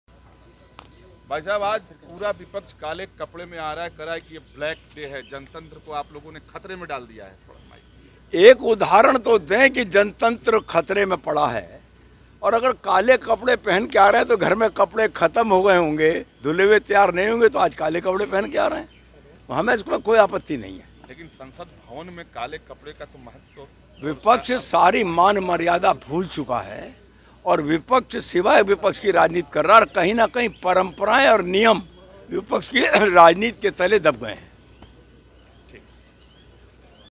काले कपडे में विपक्षी सांसद, लक्ष्मीकांत वाजपेई ने कसा तंज ।। Indiapost NEWS
काले कपडे में विपक्षी सांसद, लक्ष्मीकांत वाजपेई ने कसा तंज
About us:-